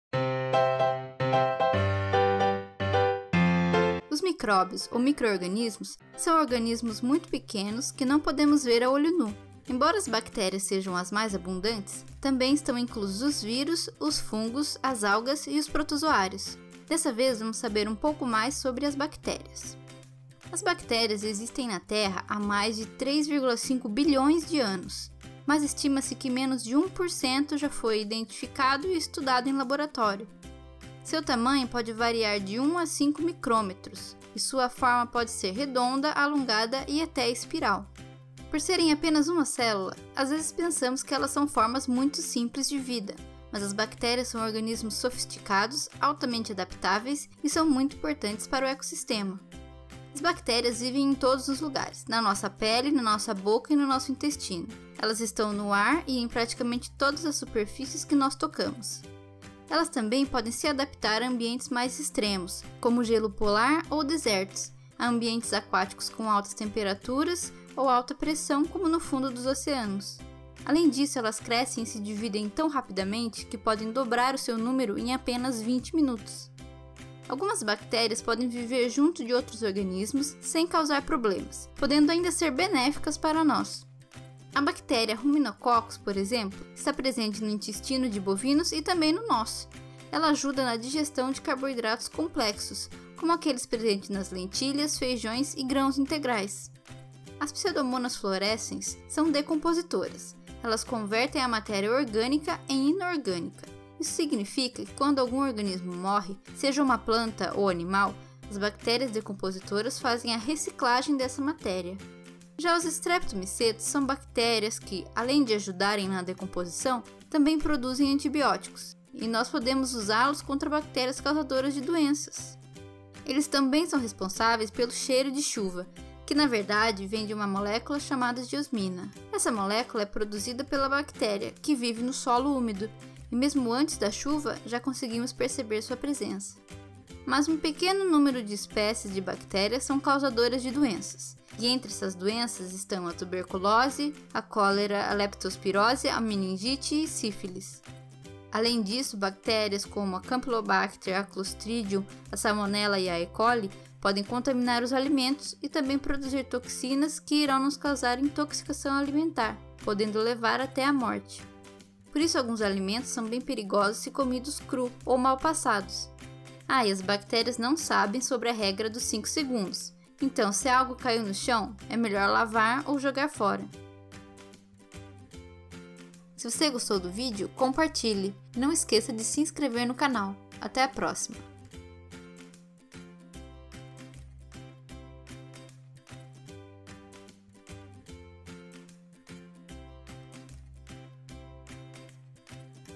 Bactérias: o que são? onde vivem? o que fazem? InstanteBiotec 63
Os micróbios, ou micro-organismos, são organismos muito pequenos que não podemos ver a olho nu. Desta vez, vamos saber um pouco sobre as bactérias.

Roteiro e apresentação: Bárbara
Pesquisa e apoio: Pablo

Referências:
Introduction to Biotechnology, 3rd edition - William J. Thieman, Michael A. Palladino.